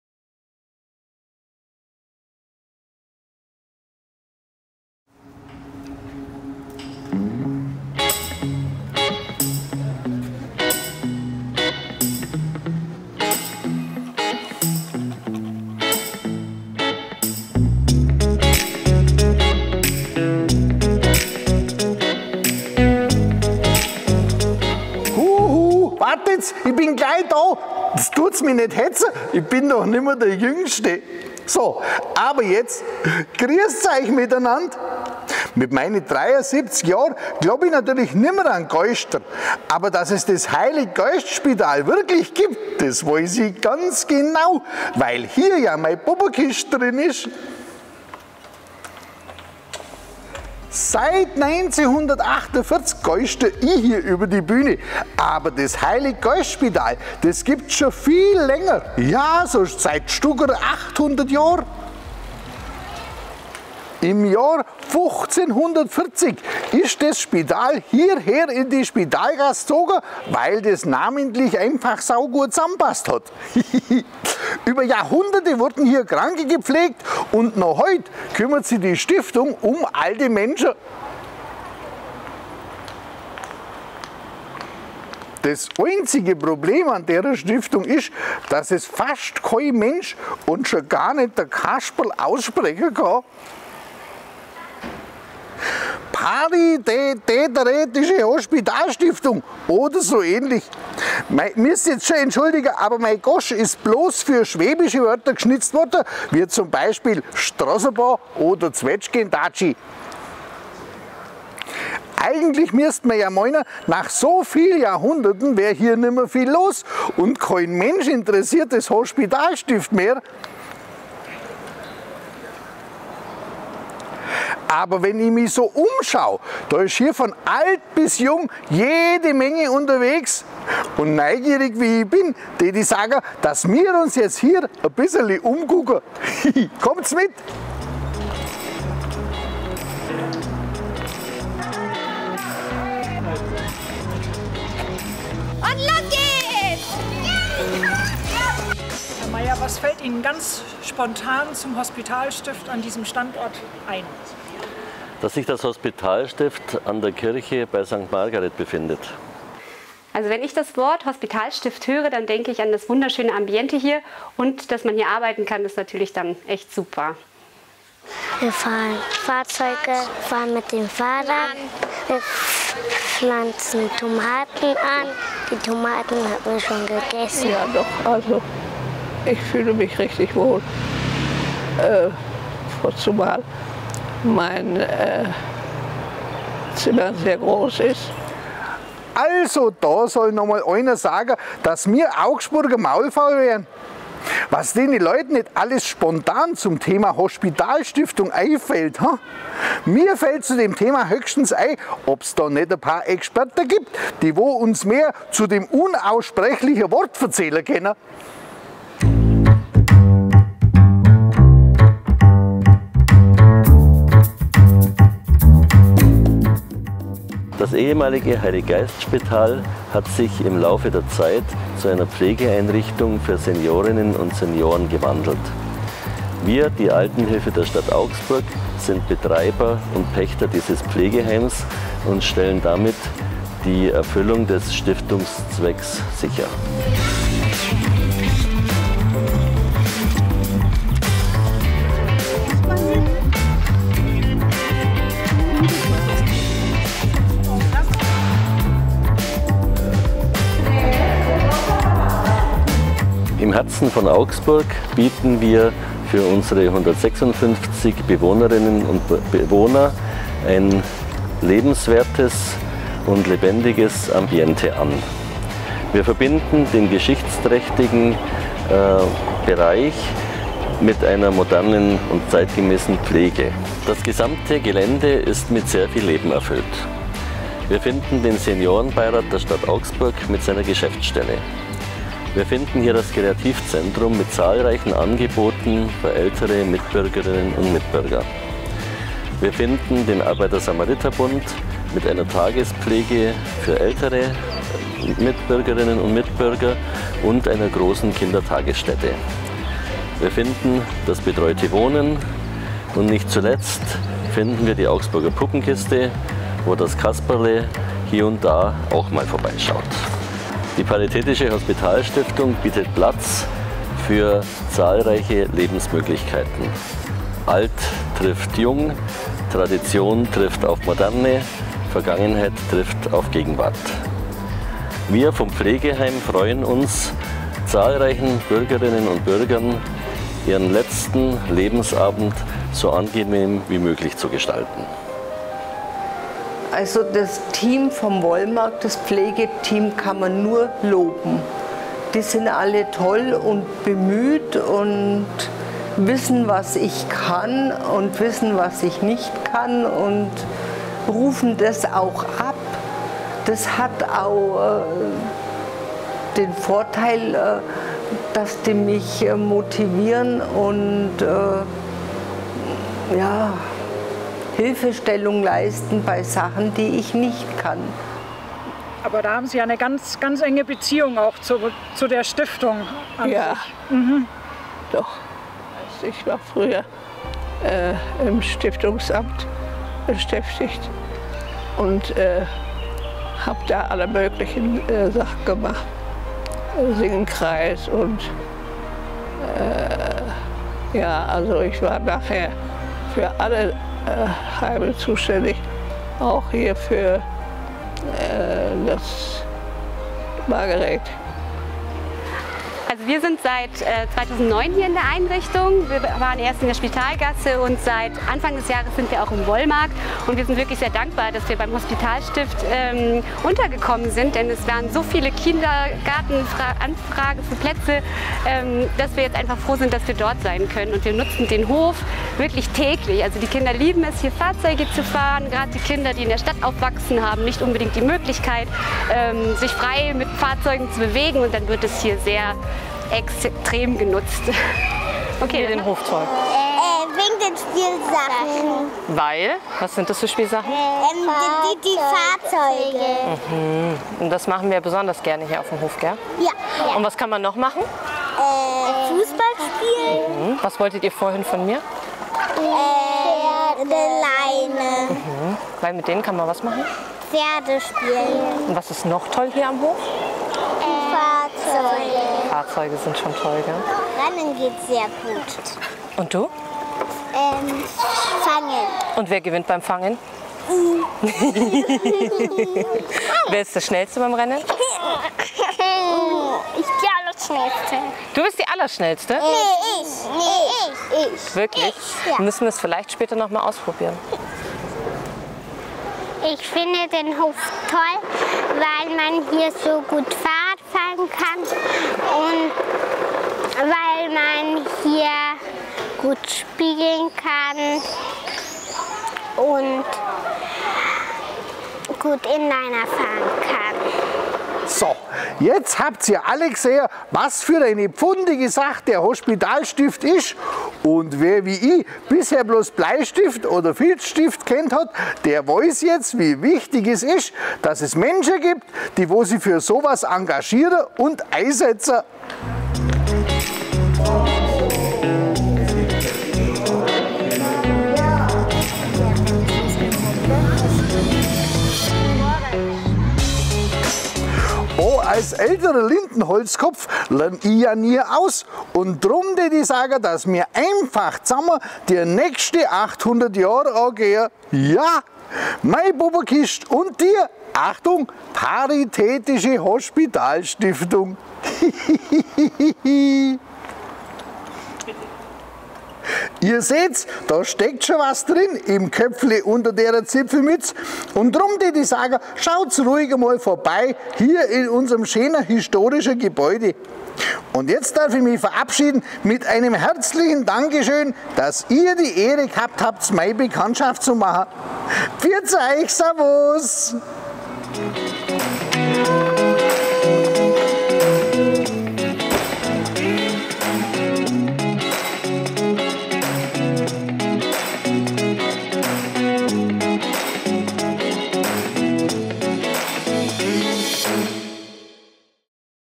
Kasperl und der Hospitalstift – Augsburger Stiftungsgeschichte(n) Teil 4
Im vierten Teil der Serie „Augsburger Stiftungsgeschichte(n)“ geht es mit der Paritätischen Hospitalstiftung mitten in Augsburgs Altstadt um eine weitere traditionsreiche Altenhilfestiftung.

Der Kasperl aus der Augsburger Puppenkiste spaziert mit uns durch die Spitalgasse, um uns die auch noch nach mehr als 800 Jahren mit viel Leben gefüllte Stiftung vorzustellen.

▬▬ Mehr Infos  ▬▬▬▬▬▬▬▬▬▬▬

▬▬ Weitere Videos  ▬▬▬▬▬▬▬▬▬▬▬

▬▬ Social Media ▬▬▬▬▬▬▬▬▬▬▬▬▬▬